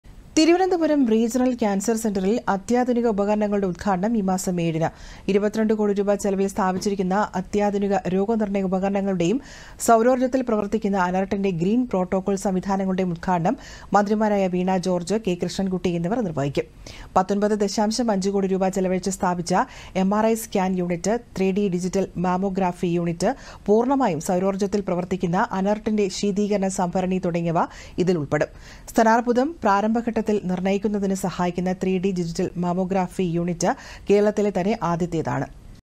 തിരുവനന്തപുരം റീജിയണൽ കാൻസർ സെന്ററിൽ അത്യാധുനിക ഉപകരണങ്ങളുടെ ഉദ്ഘാടനം ഈ മാസം 7 ന്
22 കോടി രൂപ ചെലവിൽ സ്ഥാപിച്ചിരിക്കുന്ന അത്യാധുനിക രോഗ നിർണയ ഉപകരണങ്ങളുടേയും സൗരോർജ്ജത്തിൽ പ്രവർത്തിക്കുന്ന അനെർട്ടിന്റെ ഗ്രീൻ പ്രോട്ടോകാൾ സംവിധാനങ്ങളുടേയും ഉദ്ഘാടനം മന്ത്രിമാരായവീണാ ജോർജ്ജ്, കെ.കൃഷ്ണൻകുട്ടി എന്നിവർ നിർവ്വഹിക്കും.

19.5 കോടി രൂപ ചെലവഴിച്ച് സ്ഥാപിച്ച എംആർഐ സ്കാൻ യൂണിറ്റ്, 3D ഡിജിറ്റൽ മാമോഗ്രാഫി യൂണിറ്റ്, പൂർണ്ണമായും സൗരോർജ്ജത്തിൽ പ്രവർത്തിക്കുന്ന അനെർട്ടിന്റെ ശീതീകരണ സംഭരണി തുടങ്ങിയവ ഇതിൽ ഉൾപ്പെടും.

സ്തനാർബുദം പ്രാരംഭ ഘട്ടത്തിൽ നിർണയിക്കുന്നതിന് സഹായിക്കുന്ന 3D ഡിജിറ്റൽ മാമോഗ്രഫി യൂണിറ്റ് കേരളത്തിലെ തന്നെ ആദ്യത്തേതാണ്.